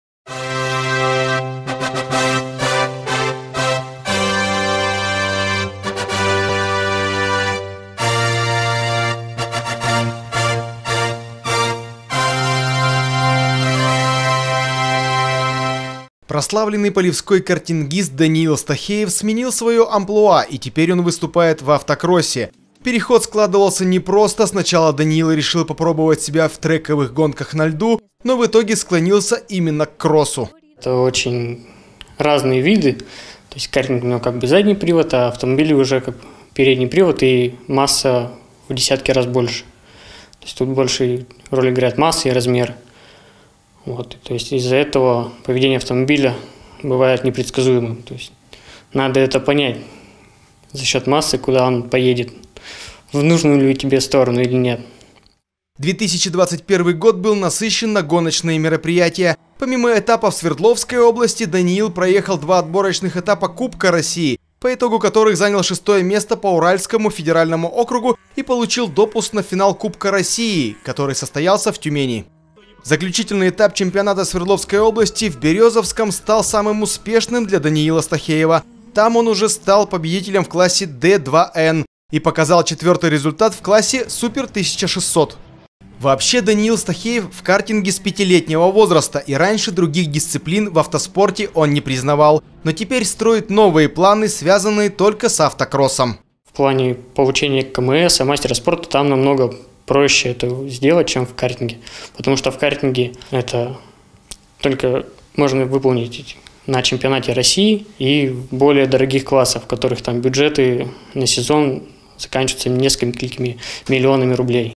Даниил Стахеев - финалист премии "Человек года - 2021" в номинации "Спорт"
Даниил Стахеев, картингист, воспитанник Спортшколы ПГО
В конце октября стал победителем в классе «Д2Н» и занял четвёртое место в классе «Супер 1600» на заключительном этапе чемпионата Свердловской области по автокроссу